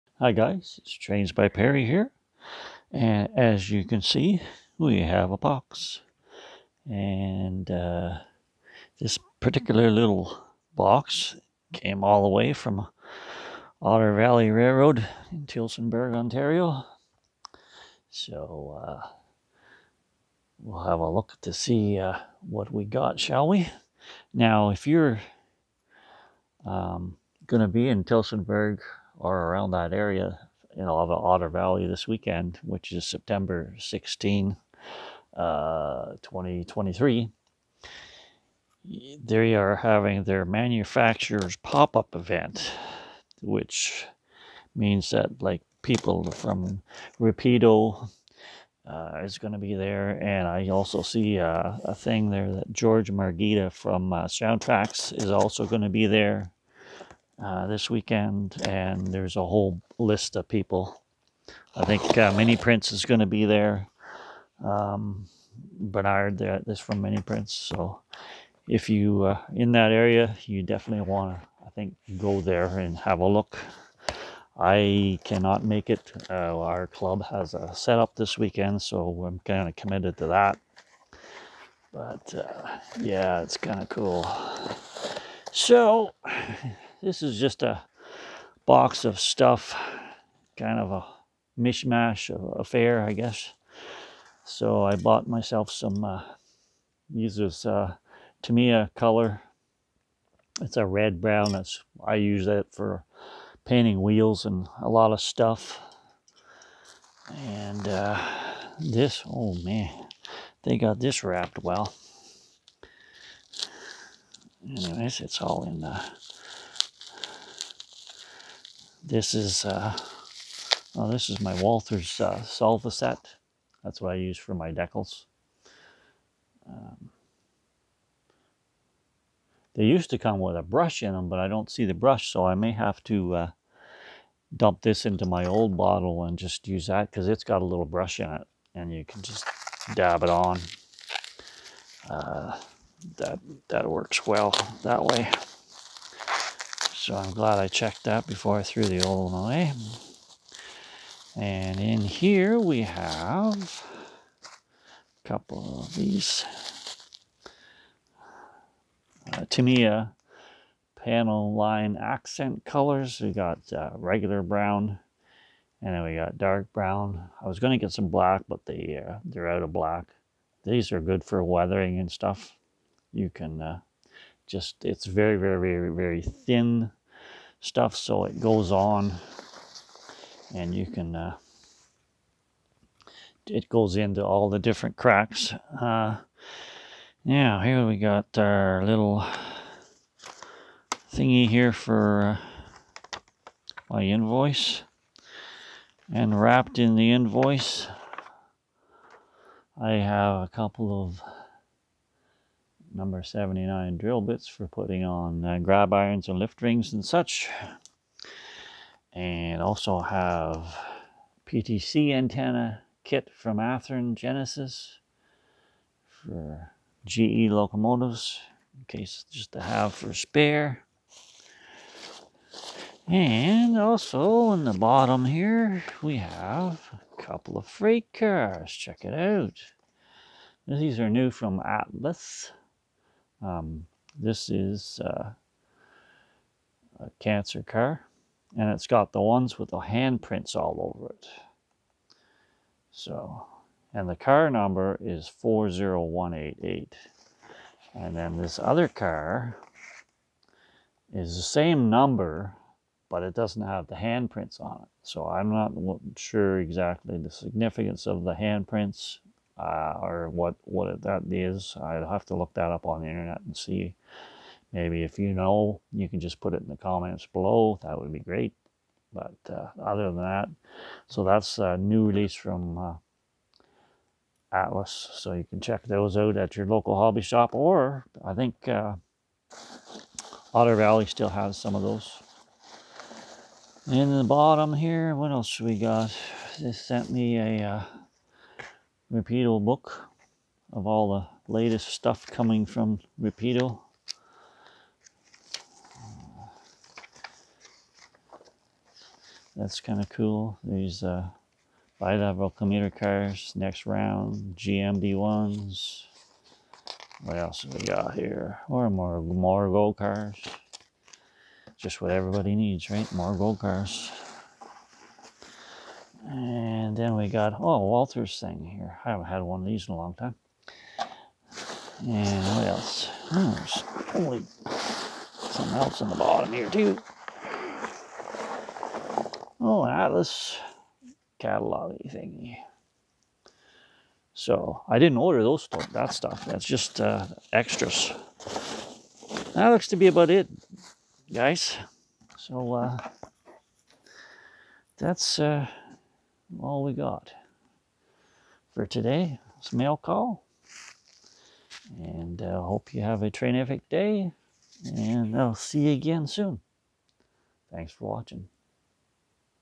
Mail Call from Otter Valley Railroad
Got this box from Otter Valley Railroad today. Want to see what's in it?